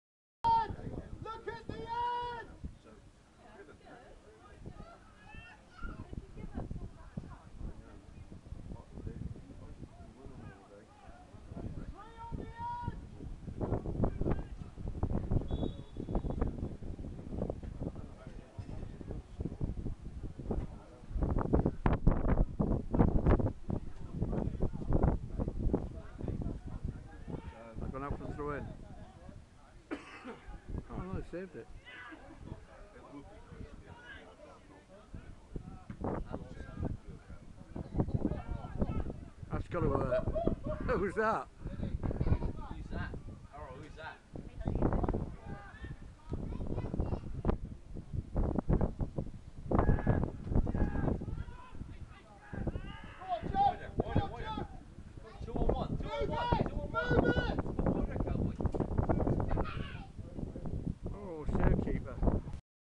ECPL Div1 Game , St Stephen 1st Team 3 v 1 Mevagissey 1st Team
Casey Laity gets a ball in the nuts,thats got to hurt.